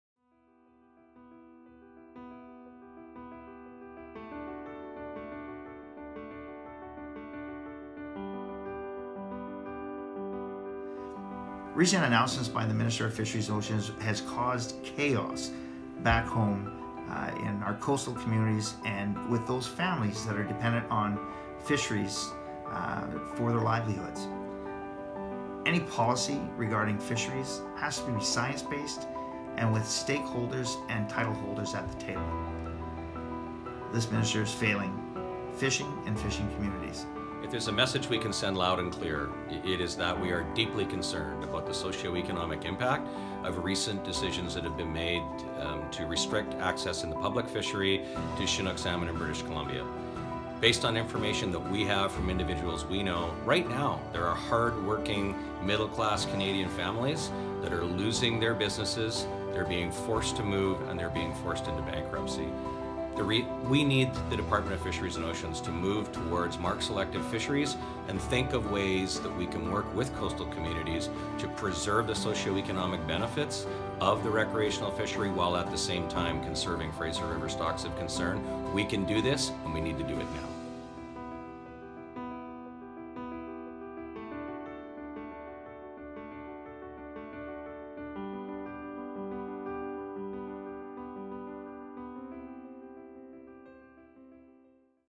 Chinook closure
Only through engagement of all stakeholders and title holders can a comprehensive policy be developed. Our goal should always be to ensure we have fish for today and fish for future generations.